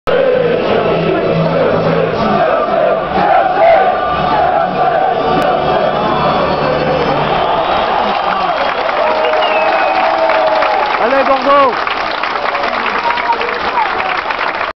Chelsea 4-0 Girondins Bordeaux 1
View from a Bordeaux fan in the home end!!
Just before Kick off. Allez Bordeaux!!